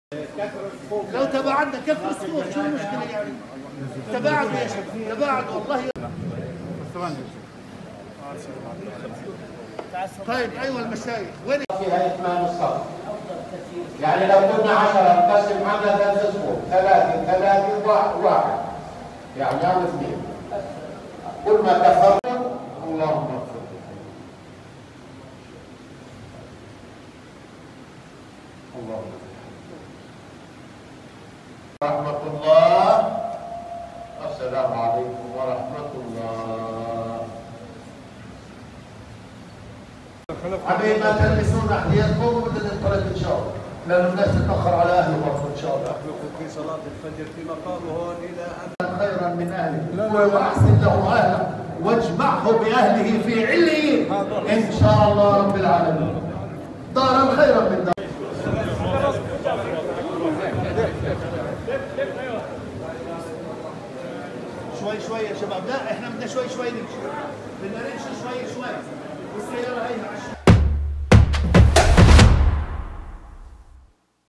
وداع حزين في نابلس للبروفيسور والأسير المحرر عصام الأشقر قبيل نقل جثمانه إلى طولكرم لتشييعه.
شاهد: وداع حزين في نابلس للبروفيسور والأسير المحرر عصام الأشقر قبيل نقل جثمانه إلى طولكرم لتشييعه.